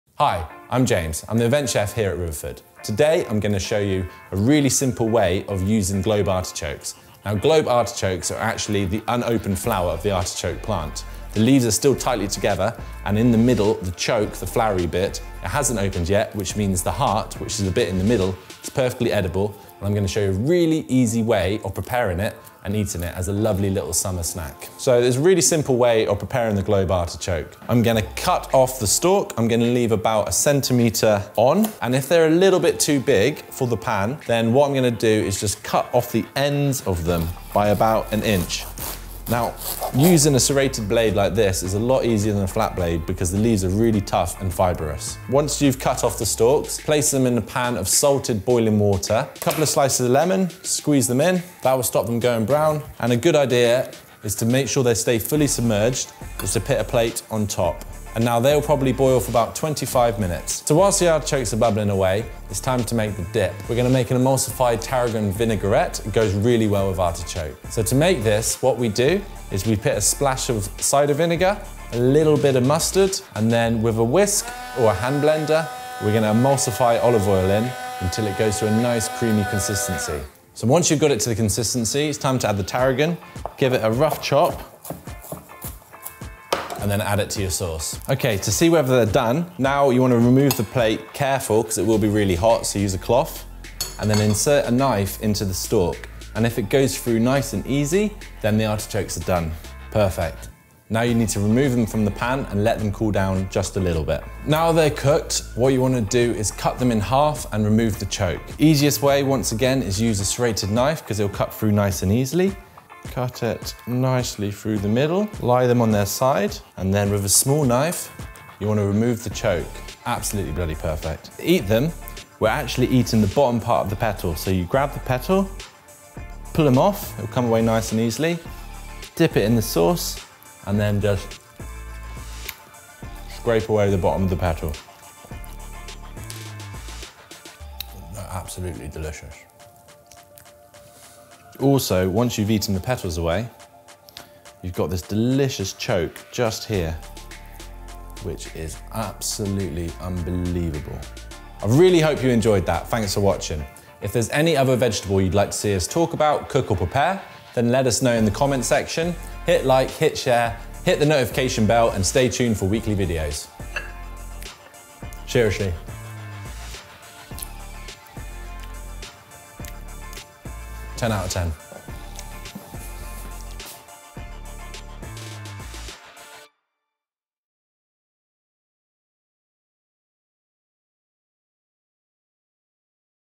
Globe Artichokes | VEG HACKS | Recipe
This week's Veg Hack is on the magnificent looking globe artichokes - one of Guy's favourites.
They stand tall and proud in the field, and provide a tasty, tactile meal in the kitchen. Here James explains how to cook them.

Please excuse the out of focus shot - we are still filming isolation style with no camera operator present.